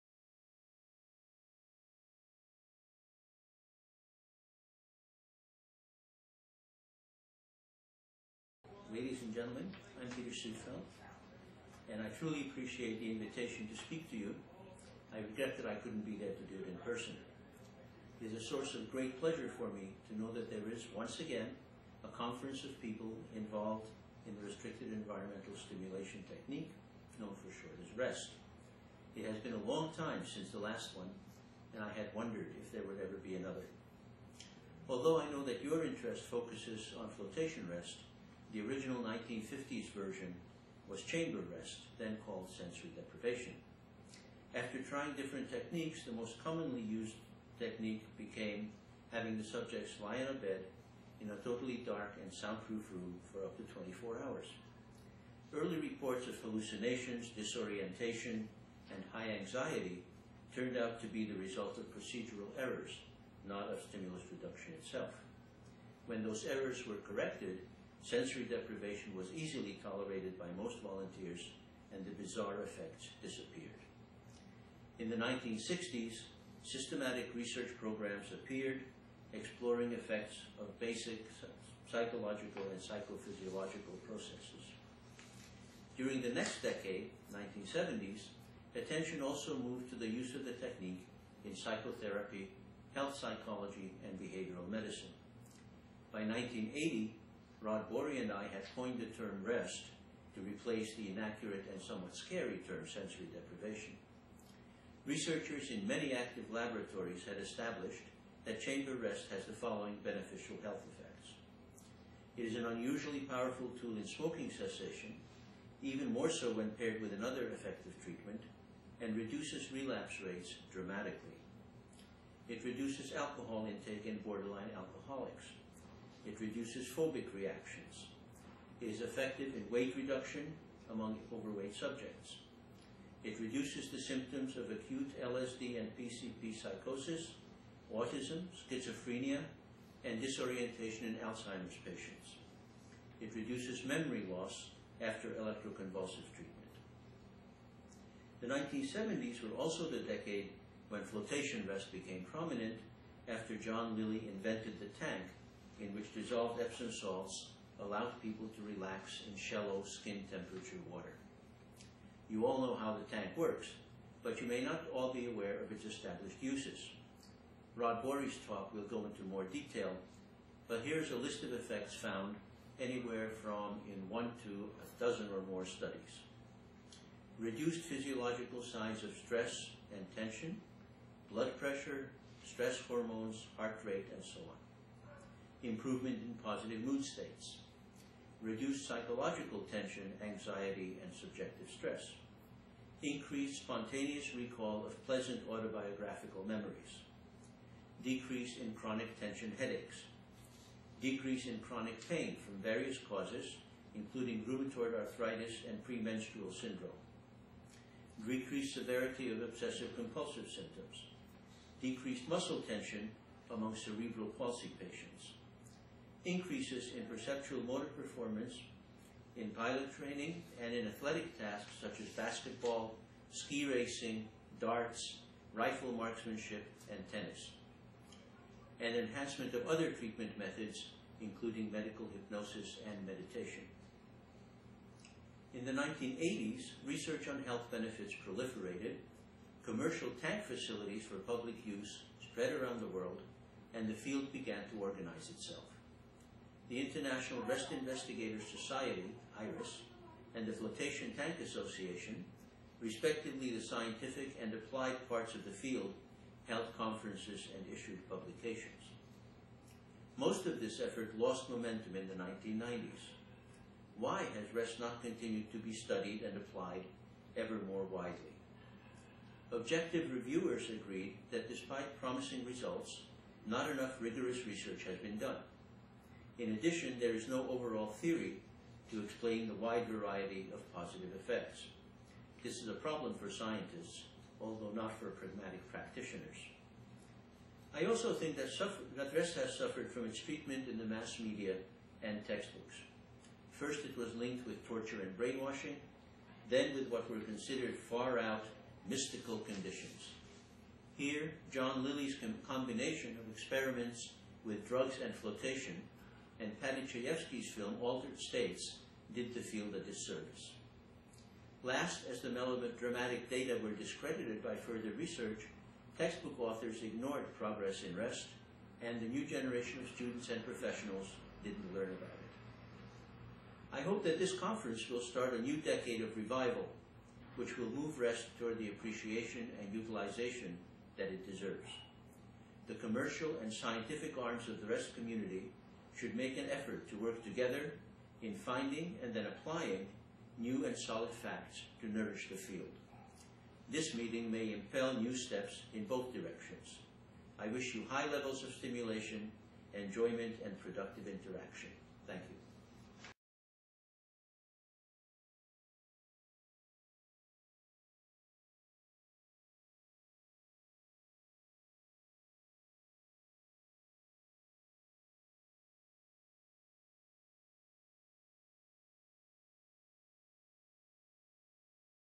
Dr Peter Suedfeld addresses the inaugural Floatation Summit, London 2010
Ladies and gentlemen I am Peter Suedfeld and I truly appreciate the invitation to speak to you and I regret that I couldn't be there to do it in person. It is a source of great pleasure for me to know that there is once again a conference of people involved in the Restricted Environmental Stimulation Technique known for short as R.E.S.T.
It has been a long time since the last one and I wondered if there would ever be another. Although I know that your interest focuses on floatation R.E.S.T. the original 1950's version was chamber R.E.S.T. and called sensory deprivation. After trying different techniques the most commonly used technique became having the subjects lie on a bed in a totally dark and sound proof room for up to 24 hours. Early reports of hallucinations, disorientation and high anxiety turned out to be result of procedural errors and not the stimulus reduction itself. When those errors were corrected, sensory deprivation was easily tolerated by most volunteers and the bizarre effect disappeared.
In the 1960's systematic research programs appeared exploring effects of basic psychological and psychophysiologicalb processes.
During the next decade the 1970's attention also moved to the use of the technique in psychotherapy, health psychology and behavioural medicine. By 1980 Rod Borrie and I had coined the term R.E.S.T. to replace in inaccurate and somewhat scary term sensory deprivation. Researchers in many active laboratories had established that chamber rest has the following beneficial health effects.
It is an unusually powerful tool in smoking cessation, even more so when paired with another effective treatment and reduces relapse rates dramatically.
It reduces alcohol intake in borderline alcoholics.
It reduces phobic reactions; it is effective in weight reduction among over weight subjects. It reduces the symptoms of acute LCD and PCP psychosis, autism, schizophrenia and disorientation in Alzheimer patients. It reduces memory loss after electroconvulsive treatment. 1970's were also the decade when floatation rest became prominent after John Lilly invented the (float) tank in which dissolved Epsom salts allowed people to relax in shallow skin temperature water.
You all know how the tank works but you may not all be aware of its established uses, Rod Borrie talk will go in to more detail, but here's a list of effects found in anything from one to a dozen or more studies. Reduced physiological signs of stress and tension, blood pressure, stress hormones, heart rate and so on. Improvement in positive mood states reduced psychological, anxiety and subjective stress. Increase spontaneous recall of pleasant autobiographical memories, decrease in chronic tension headaches, decrease in chronic pain from various causes, including rheumatoid arthritis and pre menstrual syndrome. Decrease severity of obsessive compulsive symptoms. Decreased muscle tension amongst cerebral palsy patients. Increases in perceptual motor performers in pilot training and athletic tasks such as basket ball, ski racing, darts, rifle marksmanship and tennis. And enhancement of other treatment methods including medical hypnosis and meditation.
In the 1980's research on health benefits proliferated, commercial tank facilities for public use spread around the world and the field began to organise itself. The International Rest Investigators Society (IRIS) and the Floatation Tank Association, respectively the scientific and applied parts of the field held conferences and issued publications. Most of this efforts lost momentum in the 1990's. Why has R.E.S.T. not continued to be studied and applied evermore widely? Objective reviewers agreed that despite promising results not enough rigorous research has been done. In addition, there is no overall theory to explain the wide variety of positive effects. This is a problem for scientists, although not pragmatic practitioners. I also think that R.E.S.T. has suffered from its treatment by the mass media and text books.
First it was linked with torture and brainwashing, then with what was considered "far out" mystic conditions. Here John Lilly's combination of experiments with drugs and floatation and Paddy Chayefsky's film Altered States did the field a disservice. Last as the melodramatic data were discredited by further research text book authors ignored progress in R.E.S.T. and a new generation of students and professionals didn't learn about it.
I hope that this conference with start a new decade of revival which will move R.E.S.T.  toward the appreciation and utilisation that it deserves. The commercial and scientific arms of the R.E.S.T.  community should make an effort to work together in finding and then applying new and solid facts to nourish the field. This meeting may encourage new steps in both directions. I wish you high levels of stimulation, enjoyment and productive interaction. Thank You.